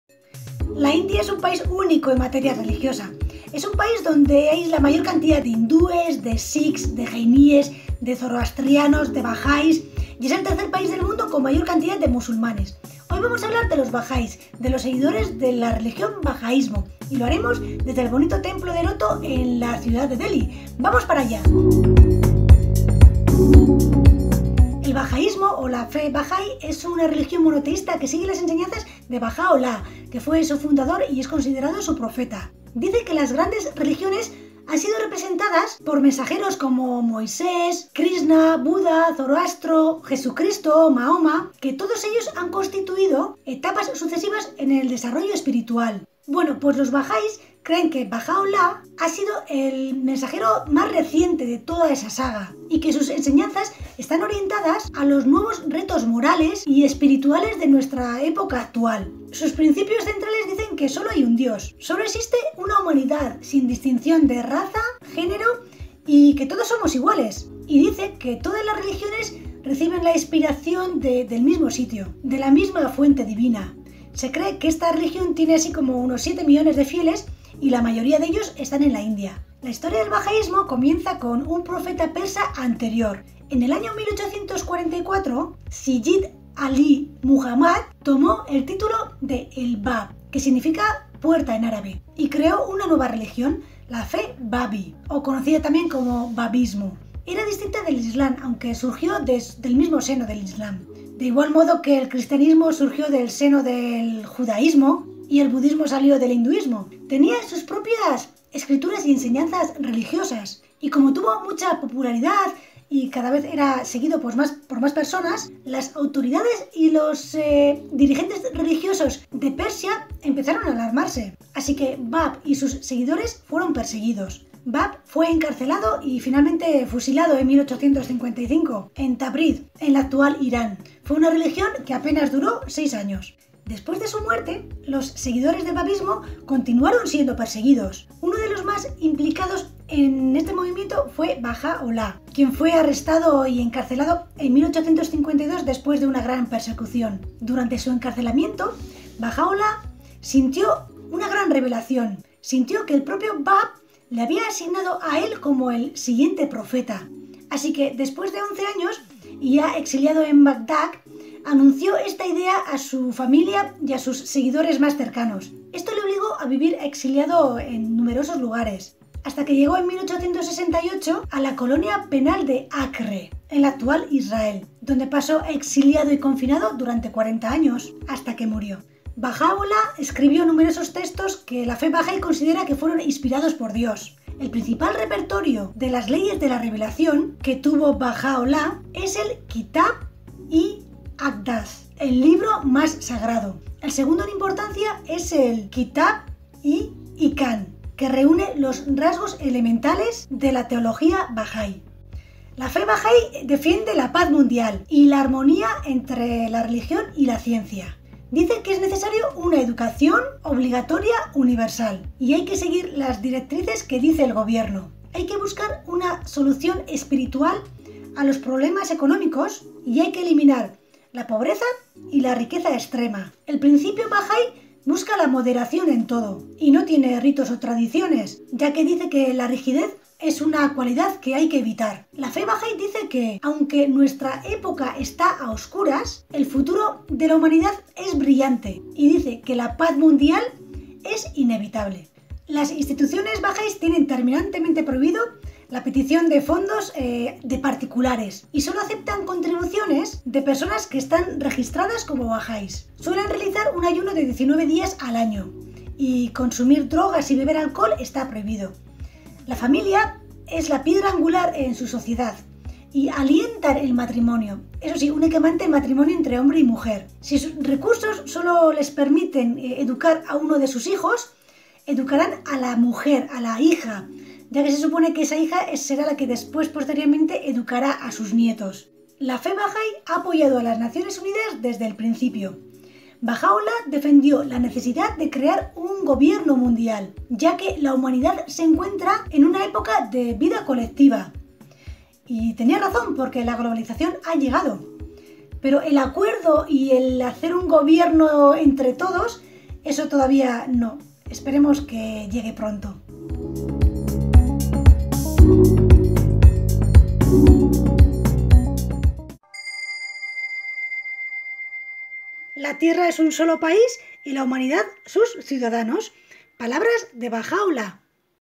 Bahaísmo o fe Bahá’í, la religión que nos anima a tener un gobierno mundial
El bahaísmo o la fe bahá’í es una religión monoteísta donde se siguen las enseñanzas de Baha´u´llah, su fundador y considerado su profeta. Dice que las grandes religiones han estado representadas por mensajeros como Moisés, Krishna, Buda, Zoroastro, Jesucristo, Mahoma, que han constituido etapas sucesivas en el desarrollo espiritual de la Civilización. Bueno pues, los bahais creen que Baha u llah ha sido el más reciente mensajero de esta saga, y que sus enseñanzas están orientadas a los nuevos retos morales y espirituales de nuestra época actual.

Sus principios centrales dicen que sólo hay un dios, sólo existe una humanidad sin distinción de raza, género, que todos somos iguales y dice que todas las grandes religiones reciben la inspiración de la misma fuente divina. Se cree que esta religión tiene unos 7 millones de fieles, y la mayoría están en la India.

La historia del bahaismo comienza con un profeta persa anterior. En el año 1844 Sayyid Ali Muhammad tomó el título de “El Bab” (La puerta en árabe) y creó una nueva religión, la Fe Babi, o Babismo. Era distinta del Islam, aunque surgió de su seno, de igual modo el cristianismo surgió del seno del judaísmo y el budismo del hinduismo. Tenía sus propias escrituras y enseñanzas religiosas. Como tuvo popularidad, y cada vez más personas se interesaban en estas nuevas enseñanzas, las autoridades políticas y religiosas de Persia se alarmaron. Así que Bab y sus seguidores fueron perseguidos. Bab fue encarcelado y finalmente fusilado en 1950 en Tabriz, en la actual Iran. Fue una religión que apenas duró 6 años.

Después de su muerte, los seguidores del babismo continuaron siendo perseguidos. Uno de los más implicados en esta religión era Baha´u´llah, quien fue arrestado y encarcelado en 1852 durante una gran persecución. Durante su encarcelamiento Baha´u´llah sintió la revelación de que él propio Bab le había señalado a él como el próximo profeta. Así que, once años después, exiliado en Bagdad anunció esta idea a su familia y a un pequeño grupo de seguidores. Esto le obligó a vivir continuamente exiliado en diferentes lugares, hasta que llegó en 1868 a la colonia penal de Acre (en la actual Israel), donde pasó exiliado y confinado 40 años, hasta que murió.

Baha´u´llah escribió numerosos textos que la fe bahai considera que fueron inspirados por Dios. El principal repertorio de las leyes de la revelación que tuvo Baha´u´llah es el Kitab-i-Aqdas “El libro más sagrado”. El segundo en importancia es el Kitab-i-Iqan que reune los rasgos elementales de la teología Bahai.

La fé bahai defiende la paz mundial, la armonía entre la religión y la ciencia. Dice que es necesario una educación obligatoria universal y hay que seguir las directrices que dice el gobierno. Hay que buscar una solución espiritual a los problemas económicos y hay que eliminar la pobreza y la riqueza extrema. El principio bahai busca la moderación en todo, y no tienen ritos o tradiciones ya que la rigidez se considera una cualidad a evitar. La fe bahai dice que, aunque nuestra época está a oscuras, el futuro de la humanidad es brillante y la paz mundial es inevitable.

Las instituciones bahais tienen terminantemente prohibido la petición de fondos a particulares, y sólo aceptan contribuciones de personas que están registradas como bahais.

Suelen realizar ayuno 19 días al año, y beber alcohol o consumir drogas está prohibido. La vida familiar es la piedra angular de su sociedad. Se alientan los matrimonios, eso si, sólo entre un hombre y una mujer. Si sus recursos permiten sólo la educación de uno de sus hijos, la hija debe ser quien la reciba ya que será la que primero eduque a sus futuros hijos.

Esta fe ha apoyado a las Naciones Unidas desde su origen. Baha´u´llah defendió la necesidad de un gobierno mundial, ya que la humanidad se encuentra en una época de vida colectiva. Y no se equivocó, la globalización ha llegado. Pero un acuerdo y un gobierno mundial todavía no.

Imágenes usadas bajo licencia CC de Yazdan2001, Rad2comet, Caspiax, Kmarina86, MichaelDPhotos y Michael Paul Colimer